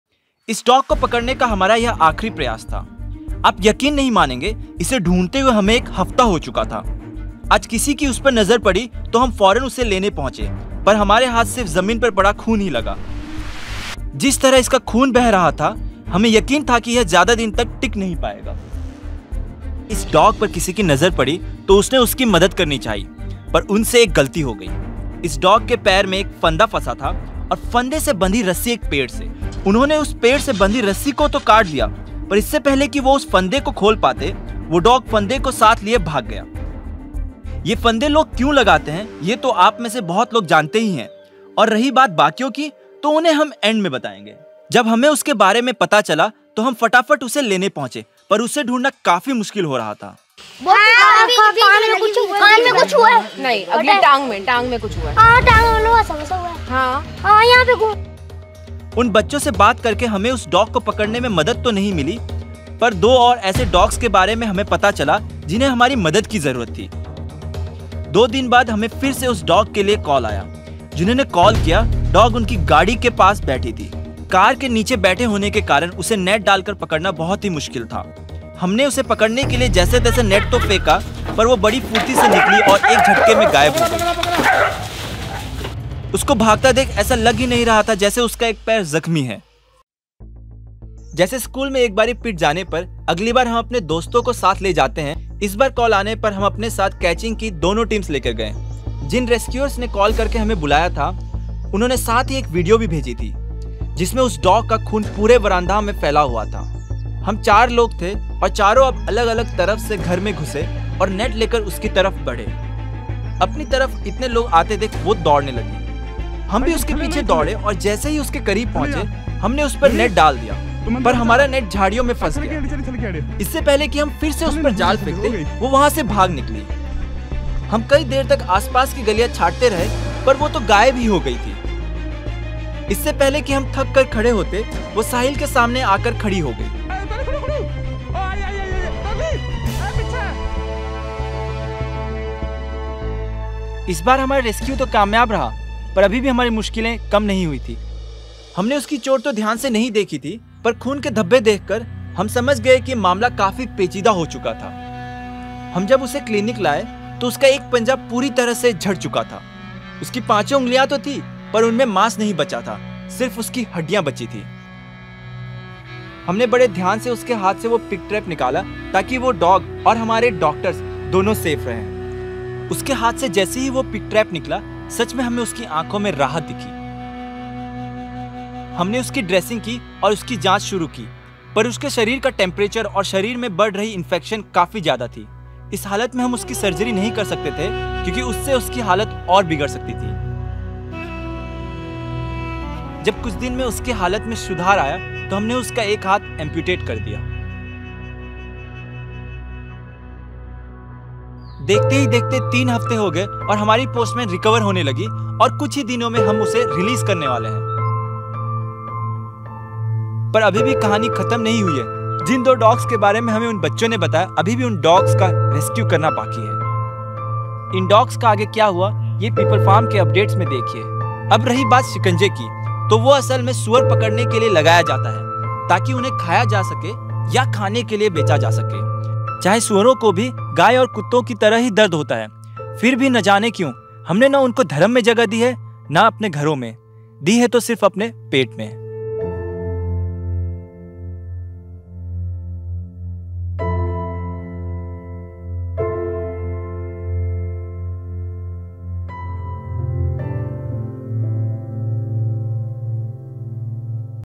Kya log, dogs ka shikaar kar rahe hain? | Dog rescue
Suar ke meat ko khaane aur use bechne ke liye kuch shikanje lagaye jaate hain jinka shikaar sirf vahi nahi balki doosre jaanwar bhi ho jaate hain.

Ab aaye din itne dogs ke cases saamne aa rahe hain ki lagta hai jaise ab log dogs ka hi shikaar karne lage hain par sochne wali baat to ye hai ki agar in traps mein fans rahe dogs ki taadad itni hai to pigs ki kitni hogi.